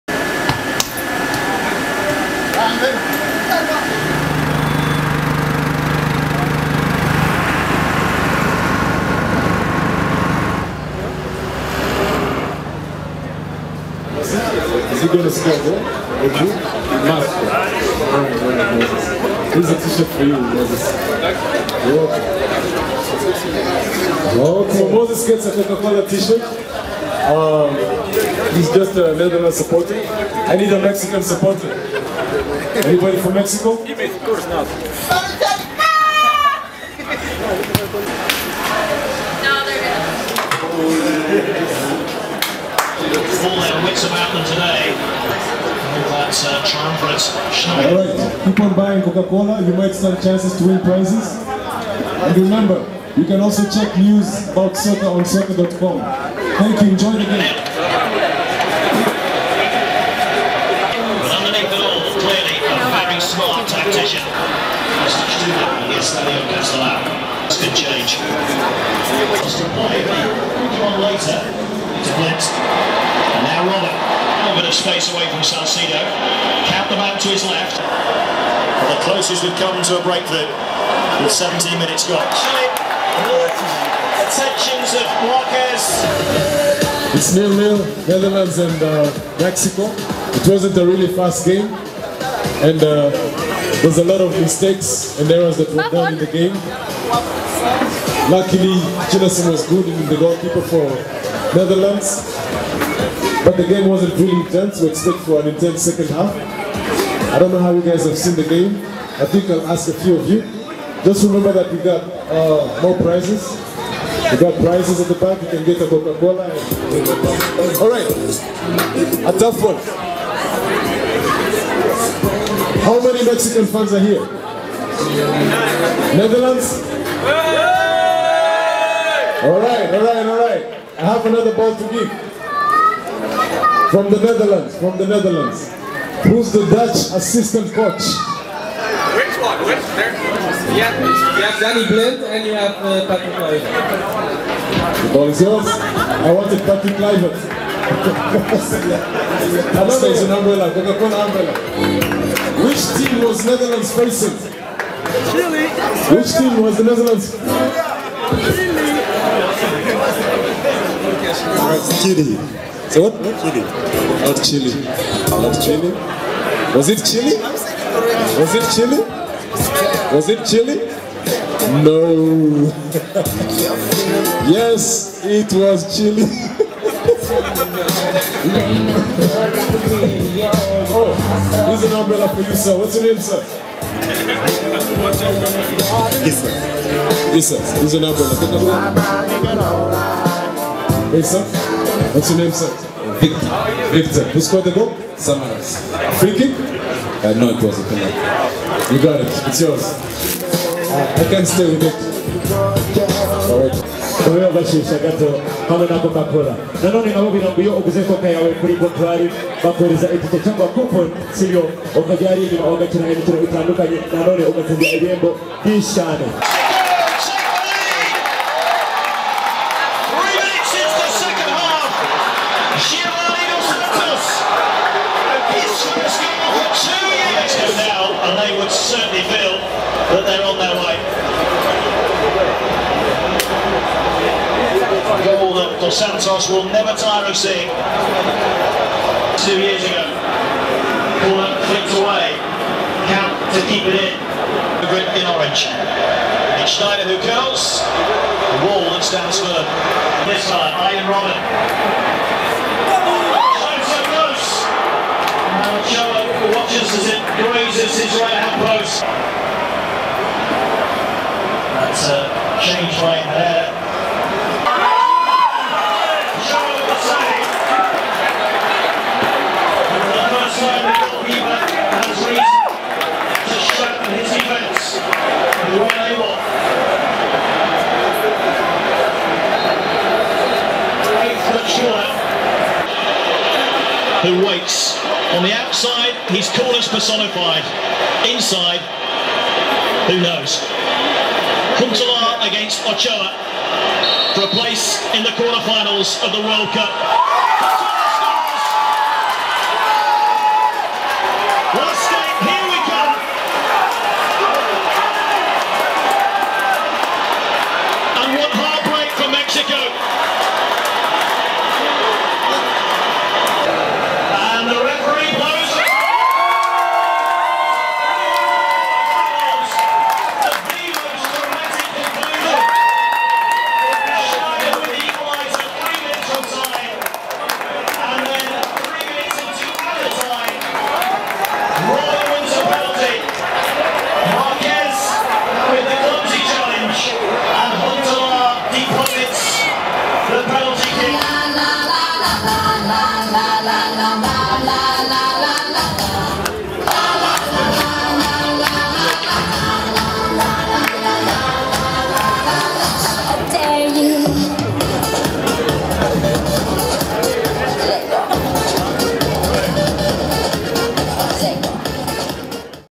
Sokkaa Hosted Coca Cola World Cup Event, Holland vs Mexico
Our team of talented, and football savvy presenters host the official World Cup event for Coca Cola, featuring the game between Holland and Mexico in the last 16 on 29/06/2014

Games were played, Coca Cola prizes were won and fun was had by all!